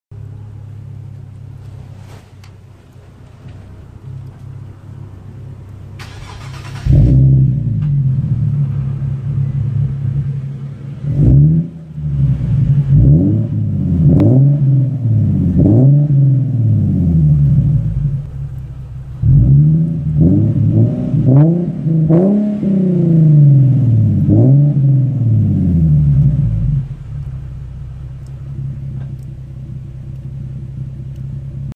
G37s coupe Topspeed axleback
Warm start. Just installed today. Im very impressed and it sounds way better in person. Best bang for your buck